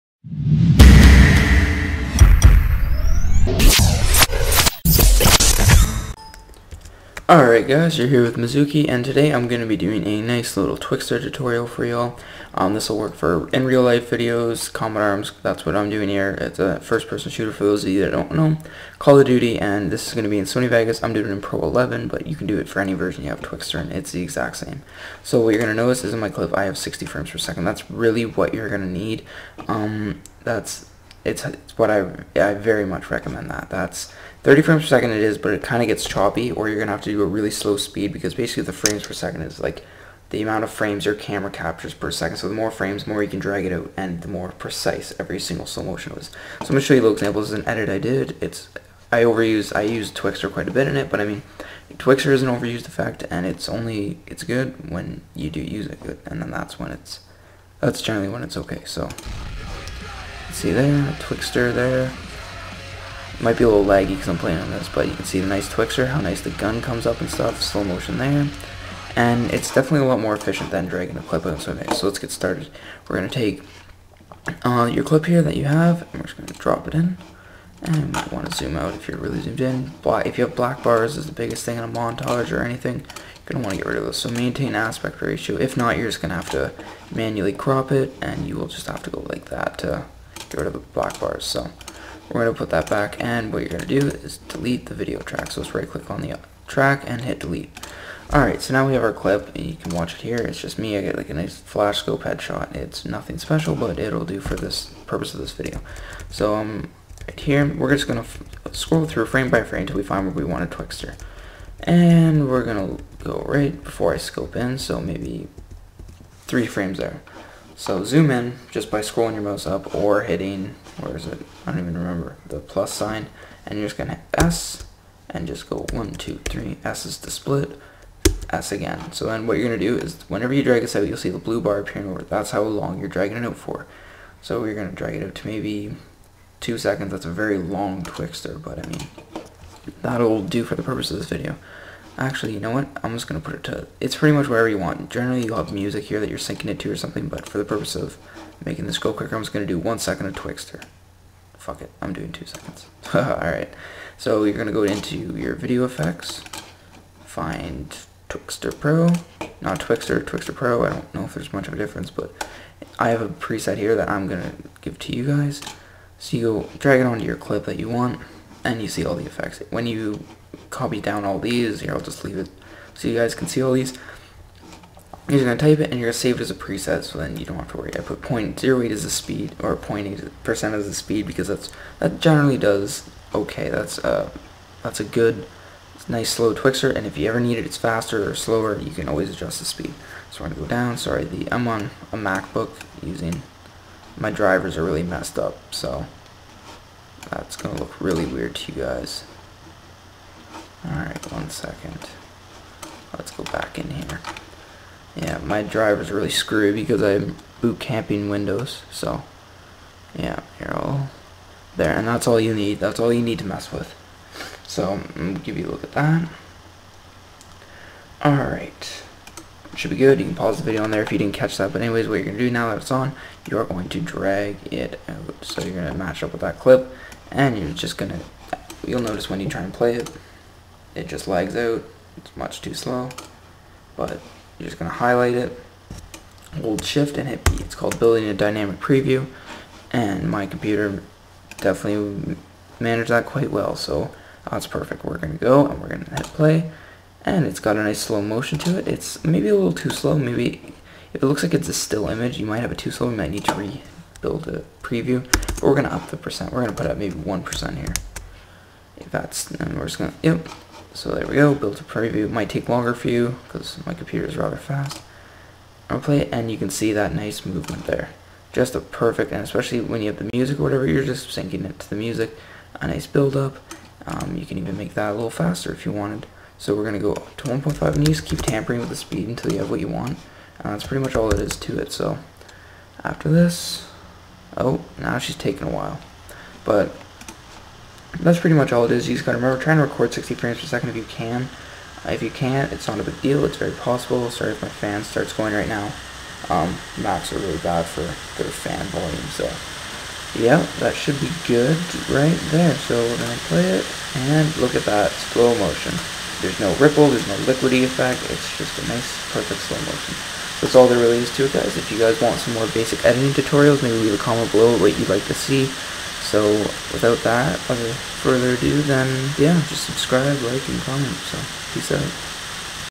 Montage Editing Tutorial #1: Twixtor - Perfect Smooth Slow Motion In Sony Vegas!
Settings are shown at 3:30

If you dont have twixtor, just youtube it and find out how to get it for free.  There are TONS of videos :)  I really hope you enjoy this, if there is anything else you guys want me to make a tutorial on, leave a comment in the description.